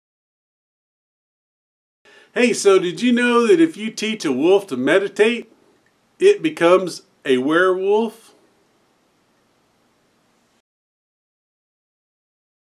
Dad Joke of the Day #1684 Meditating Wolf
When you teach a wolf to meditate: Pirate Stu's Bootyful Dad Joke of the Day #1684